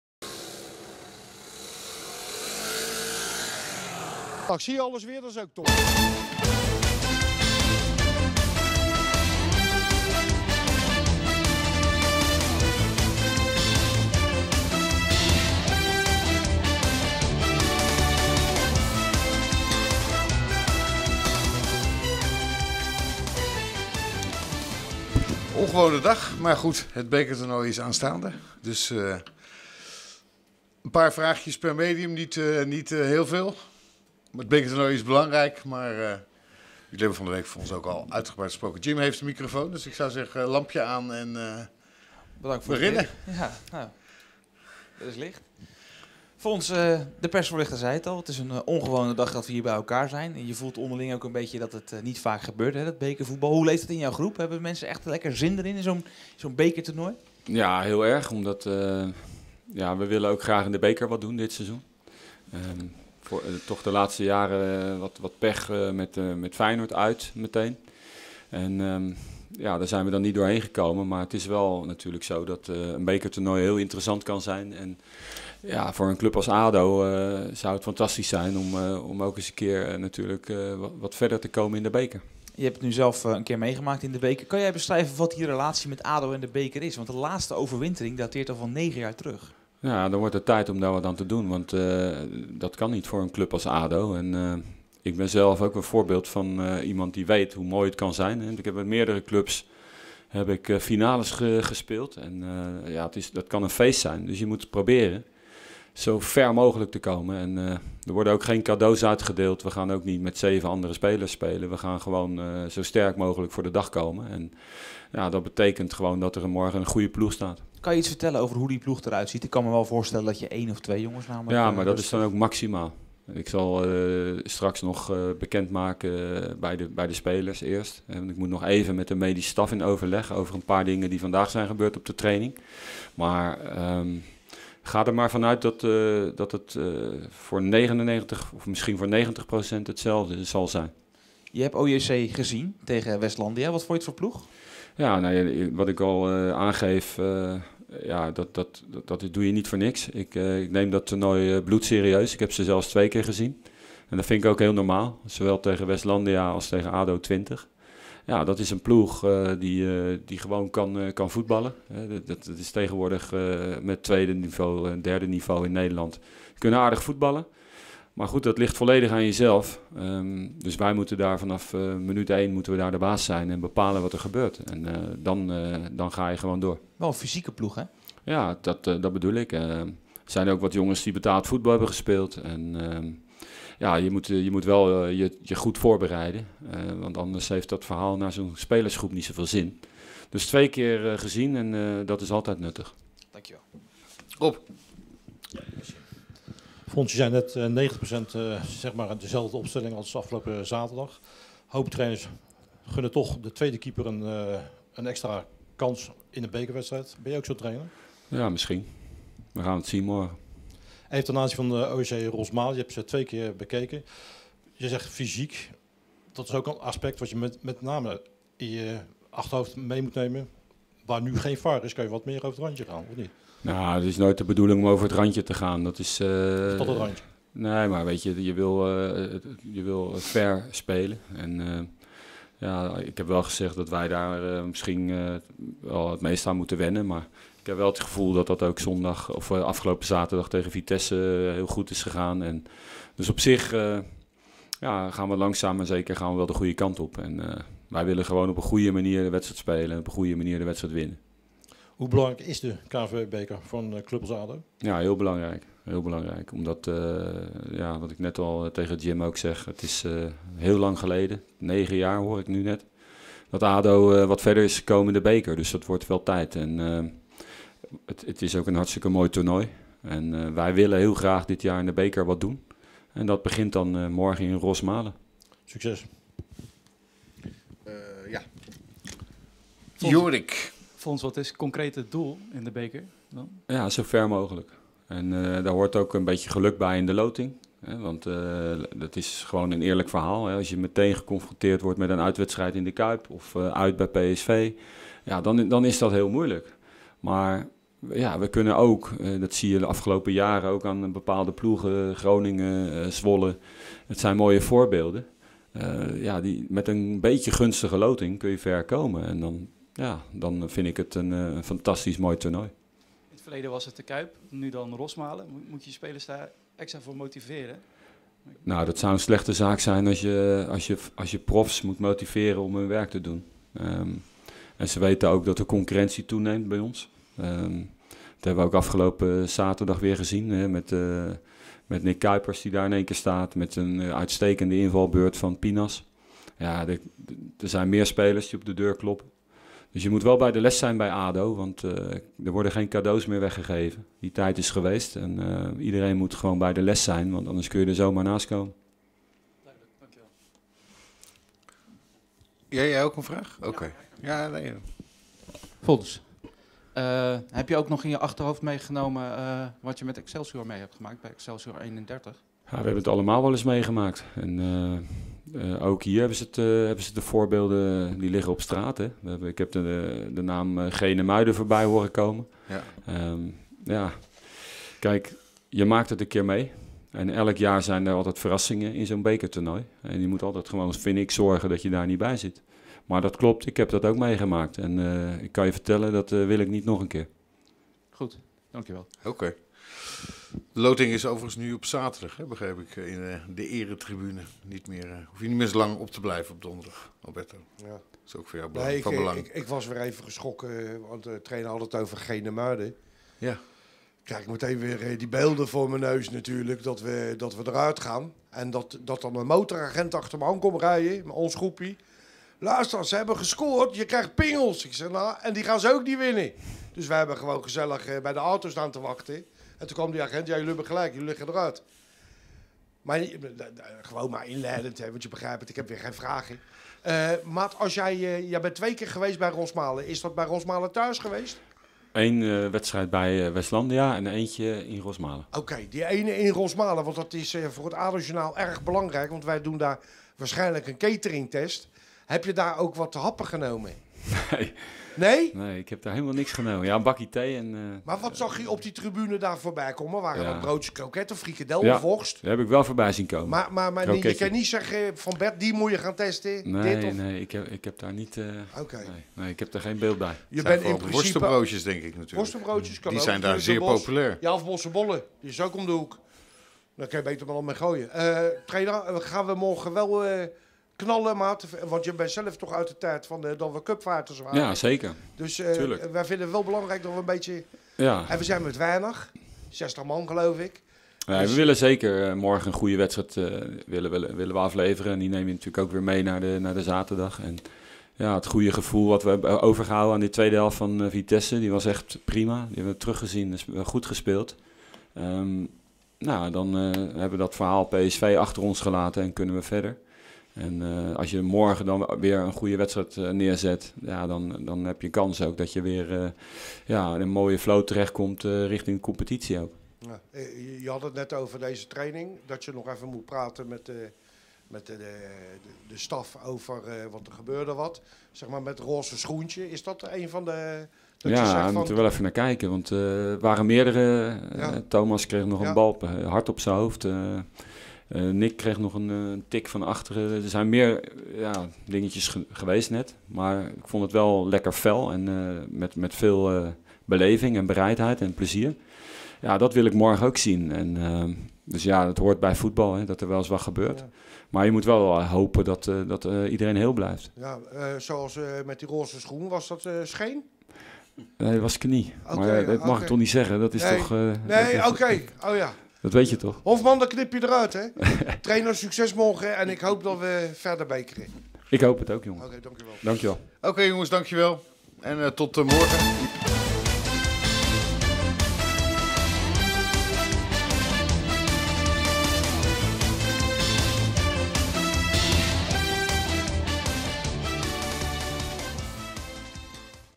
ADOjournaal met de hele Persconferentie voor OJC Rosmalen voor de Beker
Deze week beginnen we aan ons bekeravontuur.
Woensdag naar OJC Rosmalen, dus een persconferentie na de training dinsdag.
Voor de verandering eens de hele persconferentie.
Vragen van Jim (omroep west) van Rob (haaglanden voetbal) van Yorick (AD) en Wim (DHFM).
Natuurlijk had ik ook weer niks te vragen.